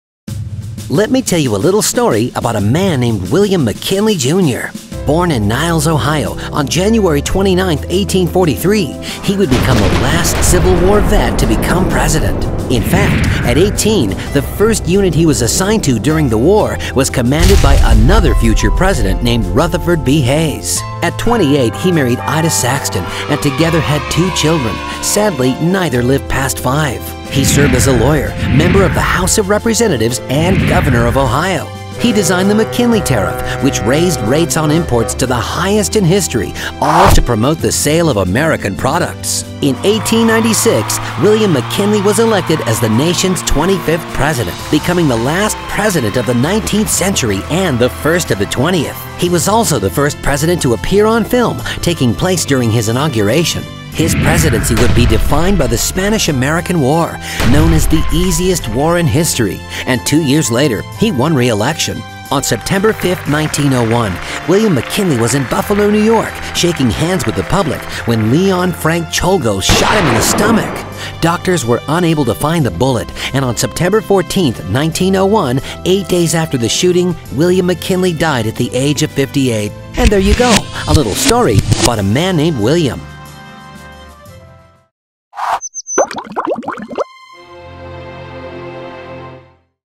William McKinley | Presidential Minute
Follow the life of William McKinley, America’s 25th President, who assumed the nation’s highest office from 1897 – 1901.   Prior to his time in office, President McKinley served as Governor of Ohio.  Enjoy this Presidential Minute with William McKinley.